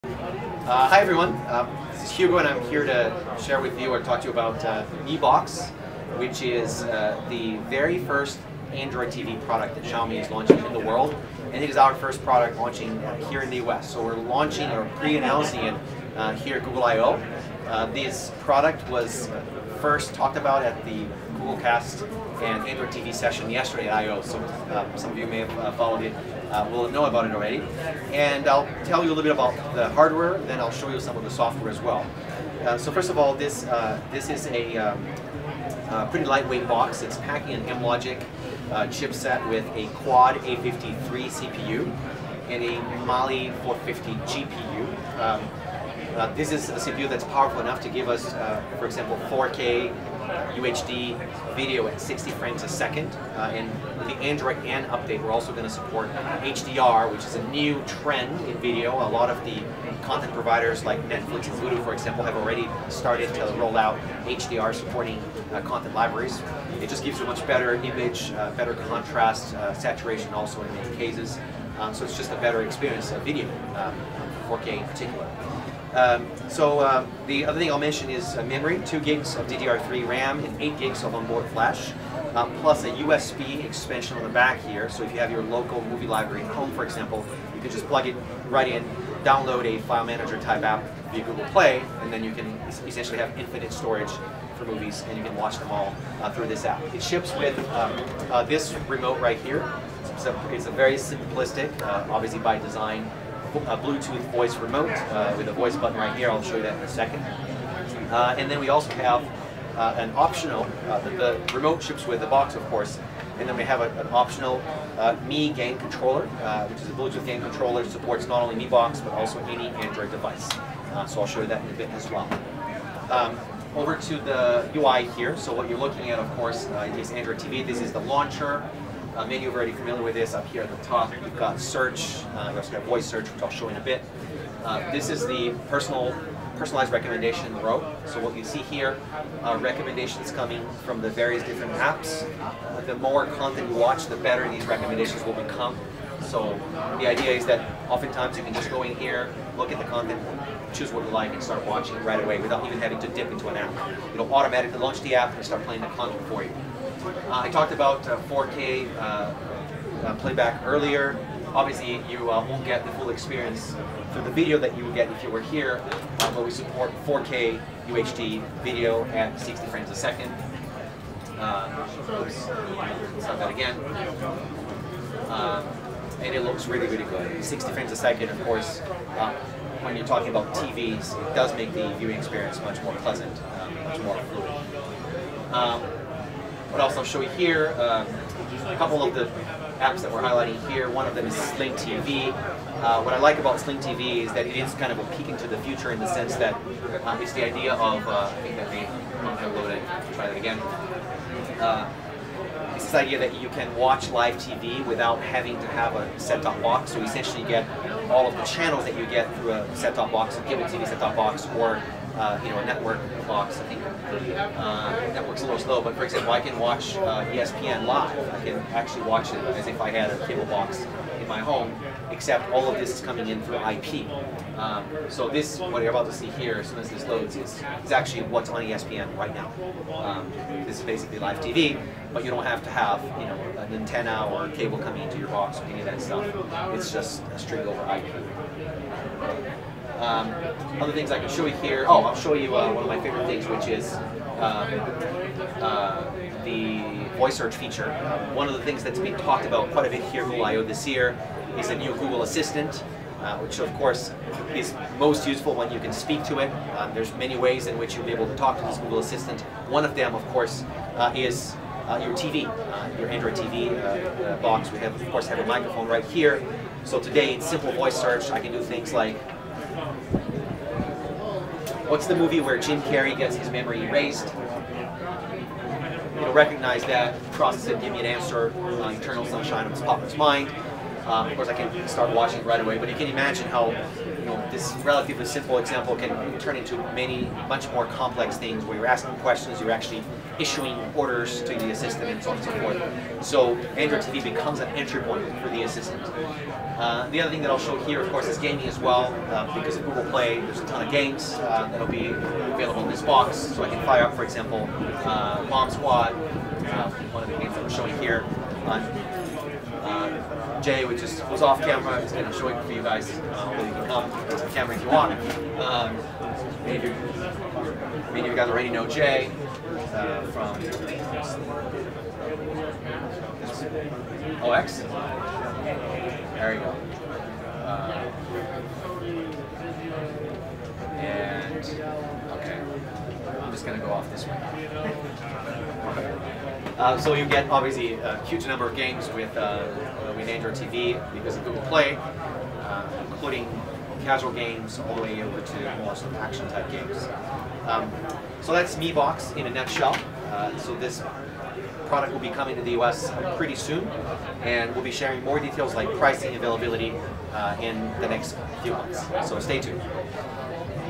Xiaomi Mi Box Demonstrated by Hugo Barra
We met with Hugo Barra at Google I/O 2016 and he had a few new things to show us, namely the Mi Box.  Xiaomi's first Android TV player ships with a little remote that's very familiar to anyone who's used an Android TV unit before, but they've also got an optional dual-stick video game controller that can be used with the unit as well.  Hugo gives us a little walkthrough of some functionality of the Mi Box and gives some pretty compelling reasons why you should seriously think of getting one when it debuts later this year.